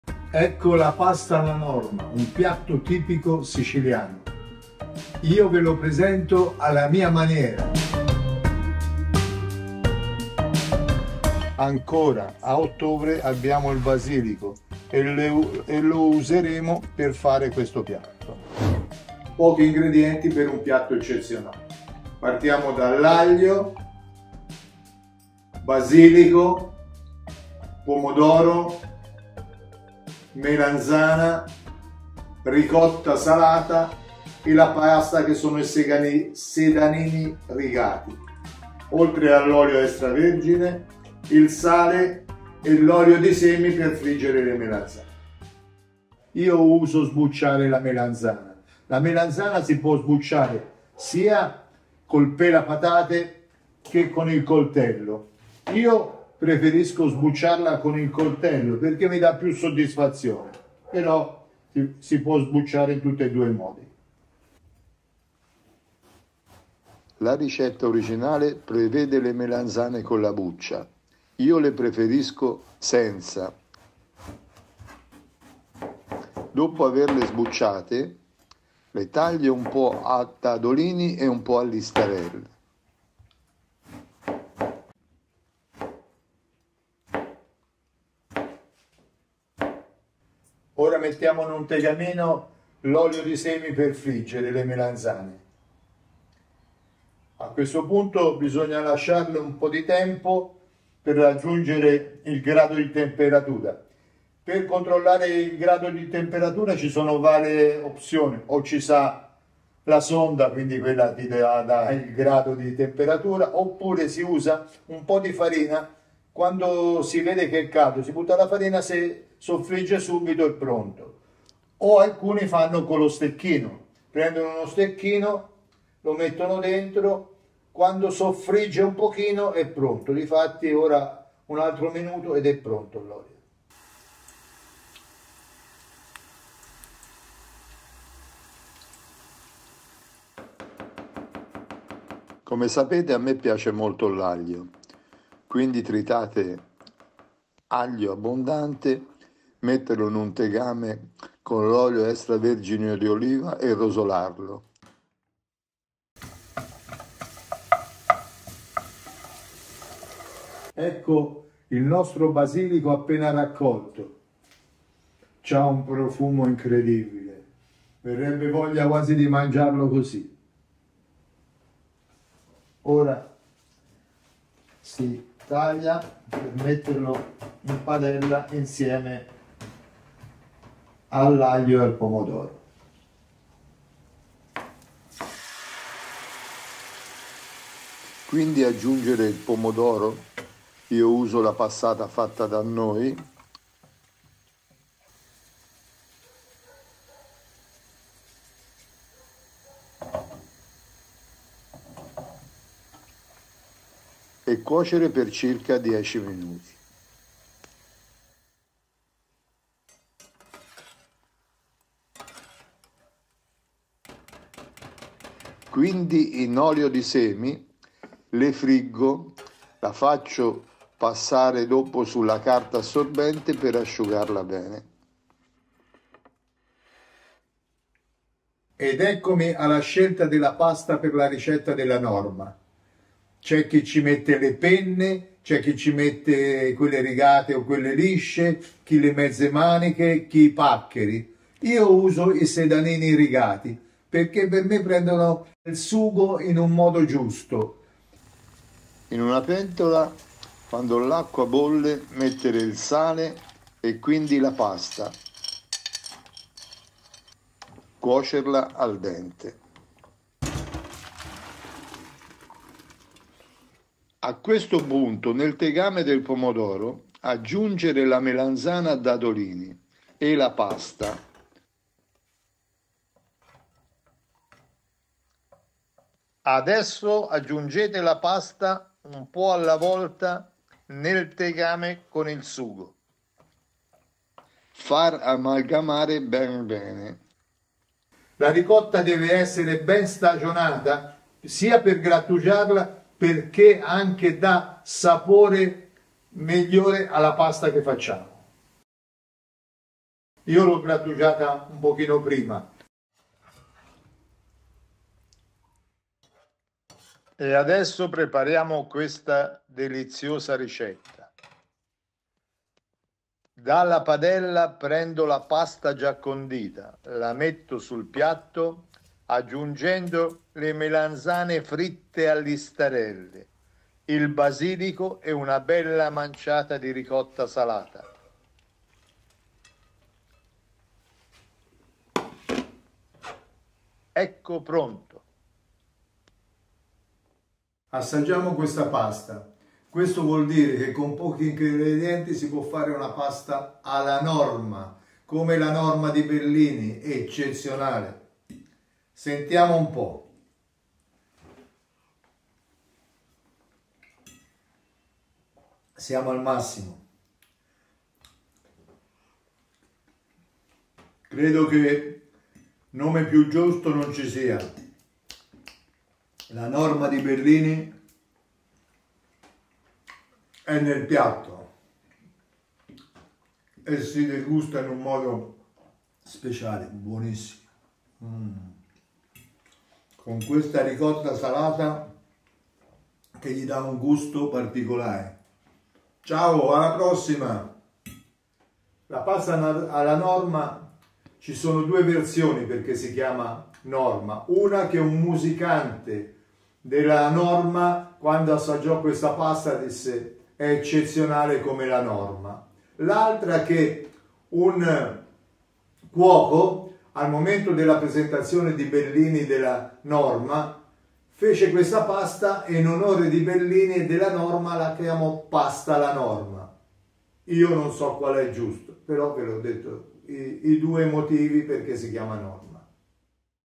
Pasta alla Norma Toscana di Aldo
Oggi faccio un bel piatto di pasta alla Norma, ricetta tipica siciliana che rivisito un po' dato che le melanzane le sbuccio prima di friggerle.
Abbiamo ancora del basilico in terrazzo quindi lo possiamo ussare prima dell'arrivo dell'inverno.

Pasta Sedanini 180 gr
Passata di Pomodoro 400gr
Melanzane 300 gr
Ricotta Salata almeno 50gr
Basilico in quantità
1 spicchio di aglio
Olio, Sale, Pepe
Olio di Girasole per friggere la melanzana

0:00 Ingredienti
0:45 Preparazione
4:08 Cuocere e Impiattare
5:37 Assaggio
6:29 La Norma?